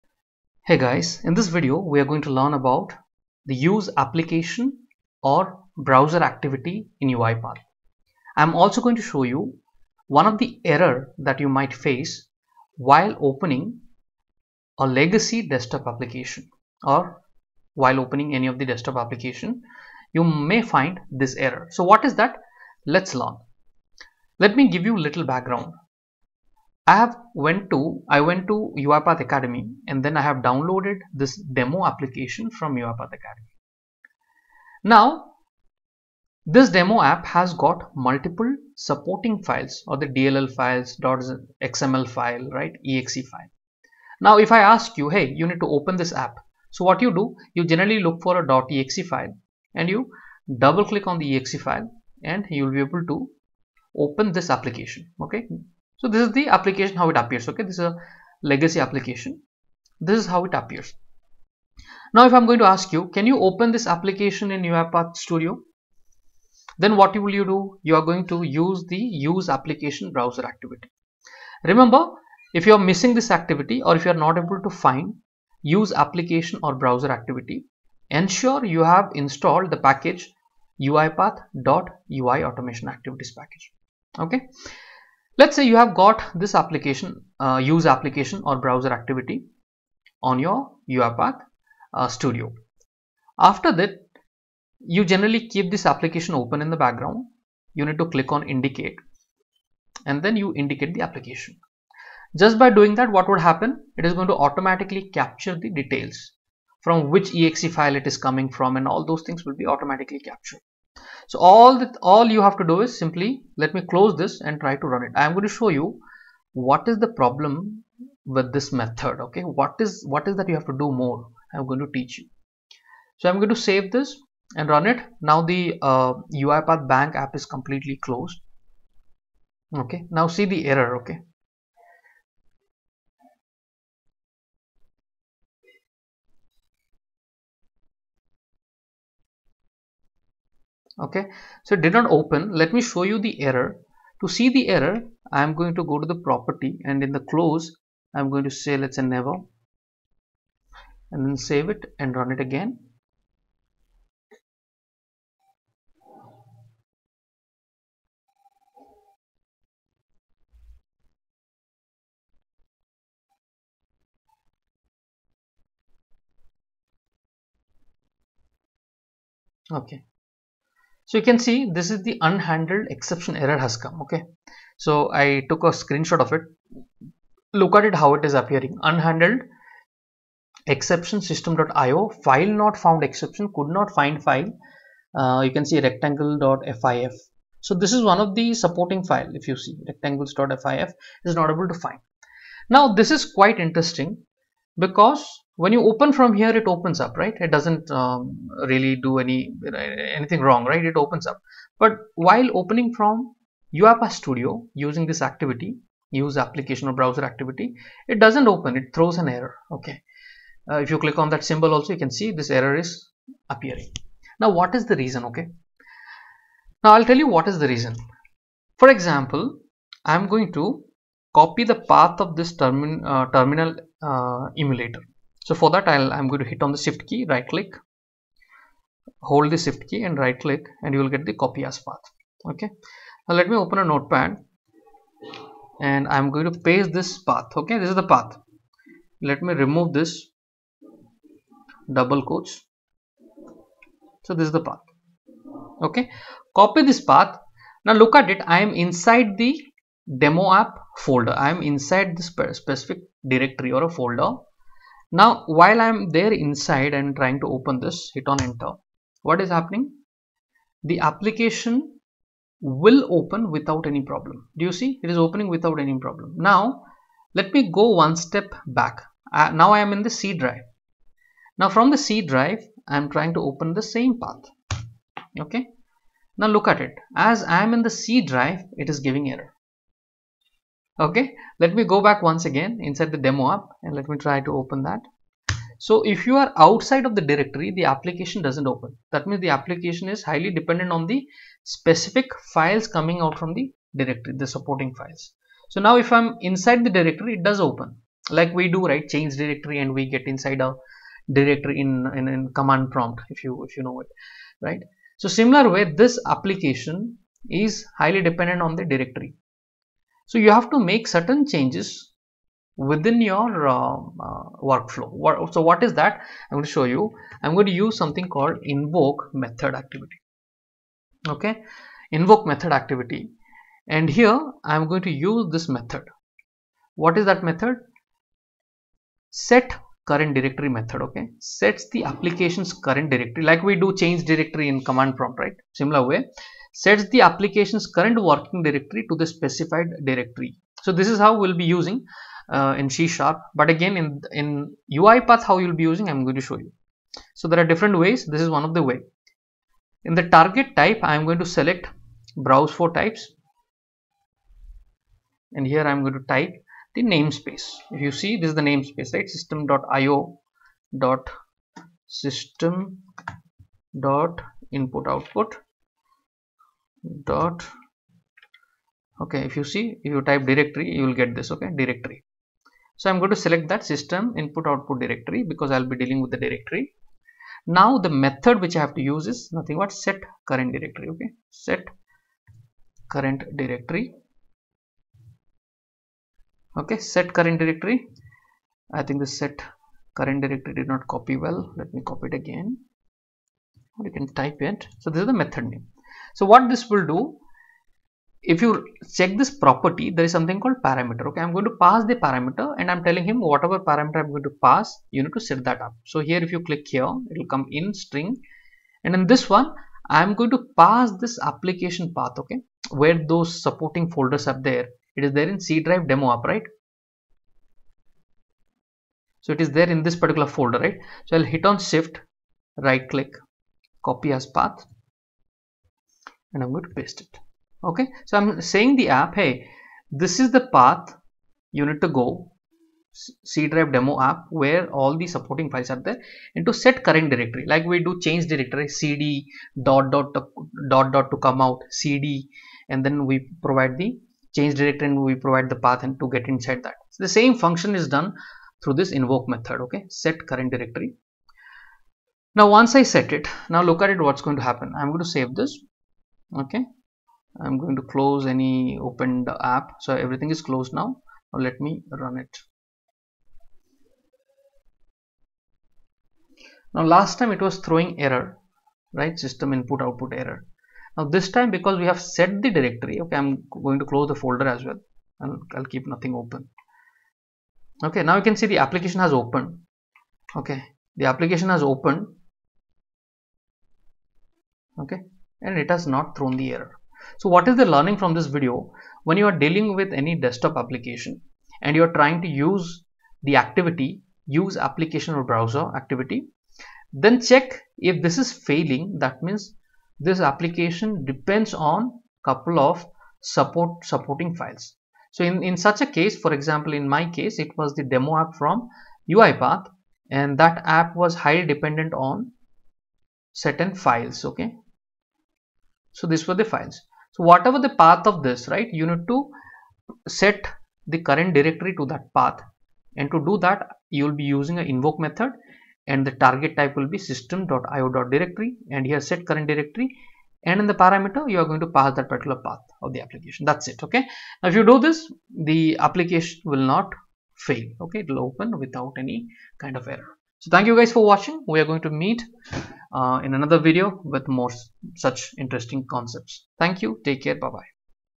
UiPath | Use Application/Browser Activity | How Do I Use the "Use Application/Browser" Activity
This video we will learn about "Use Application/Browser" in UiPath. While opening a desktop application in UiPath you may get error while setting up the UiPath Use Application or Browser activity. Let's learn.